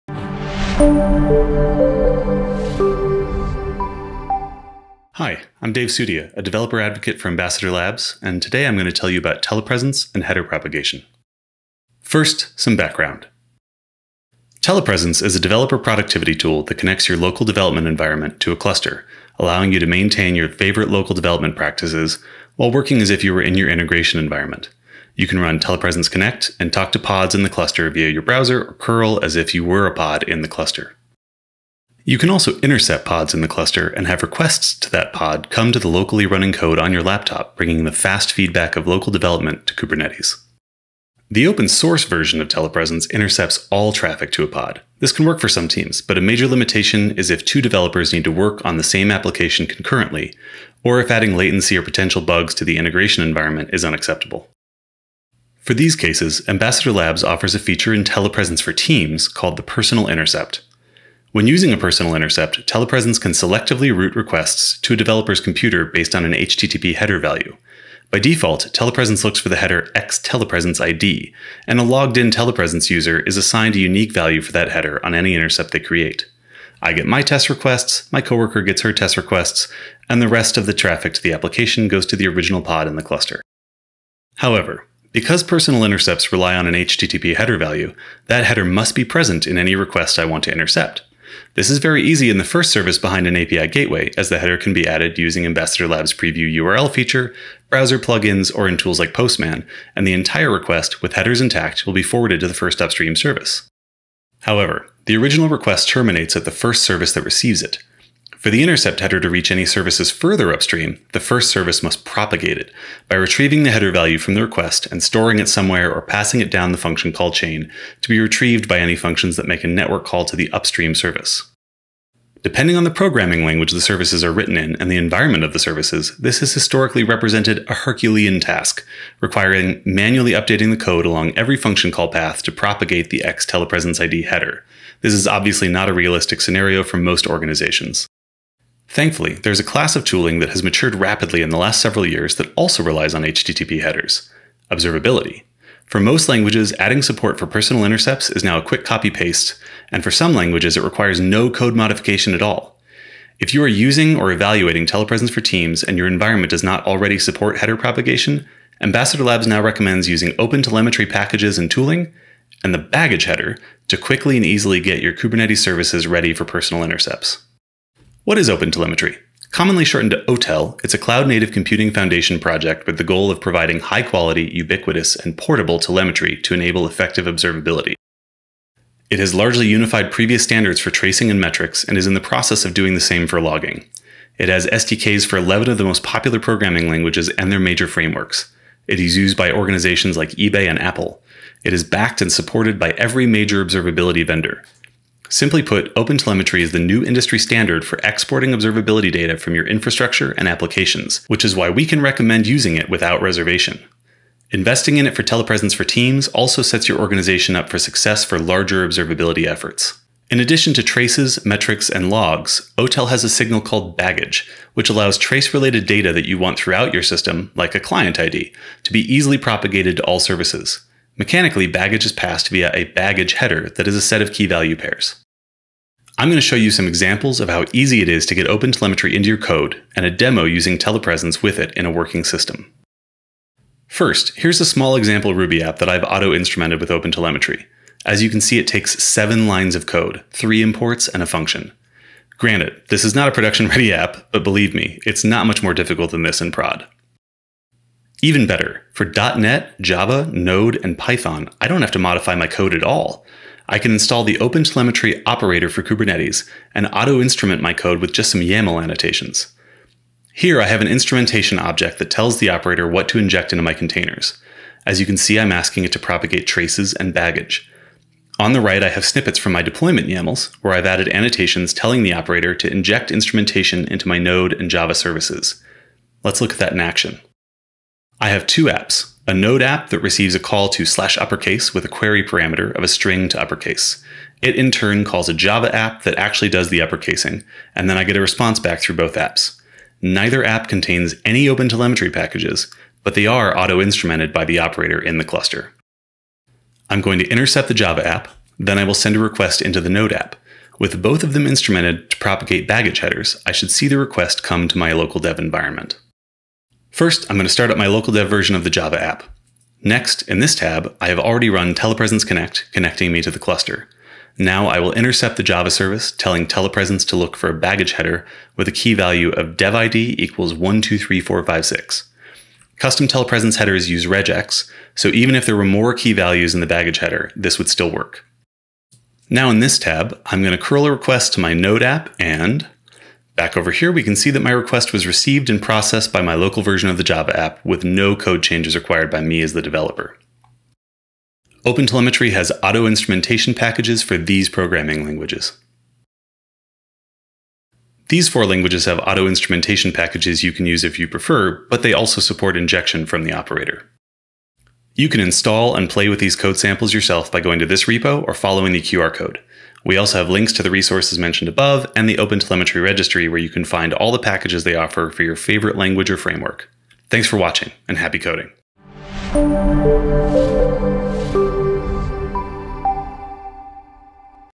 Telepresence Header Propagation
Telepresence. How to use OpenTelemetry libraries to propagate the
OpenTelemetry baggage header. Telepresence has an option to customize the HTTP header it should look for. We can customize
it to look for a specific key-value pair in the baggage header.